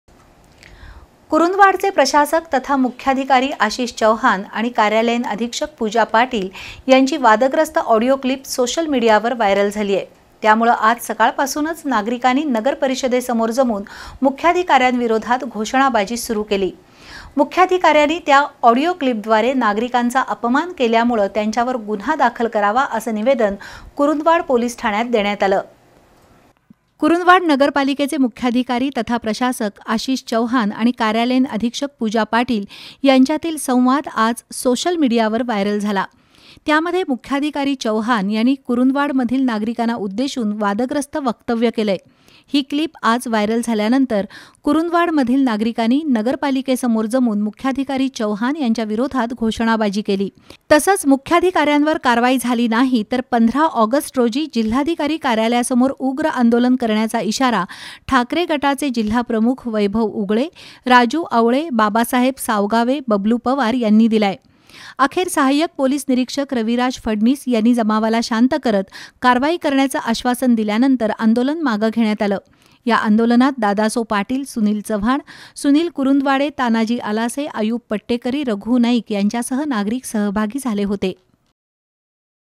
कुरुंदवाडचे मुख्याधिकारी आशिष चौहान यांची ऑडिओ क्लिप व्हायरल झाल्यानं नागरिकांमध्ये संताप
कुरुंदवाडचे मुख्याधिकारी आशिष चौहान यांची ऑडिओ क्लिप व्हायरल झाल्यानं नागरिकांमध्ये संताप, नागरपालिकेसमोर झाली घोषणाबाजी